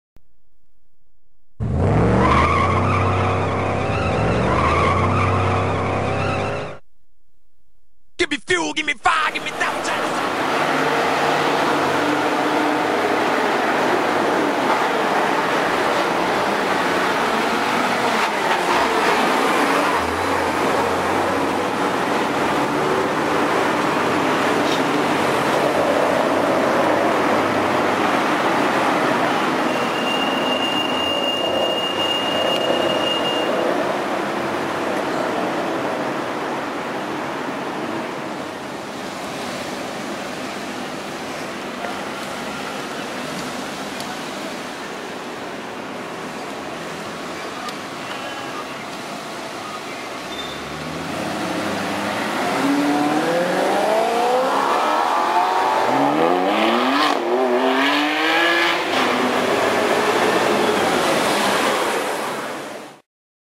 Ferrari 599GTB Fiorano na Av. Europa
Speed Brasília registrou uma Ferrari 599 GTB Fiorano saíndo da Platinuss na Av. Europa em São Paulo.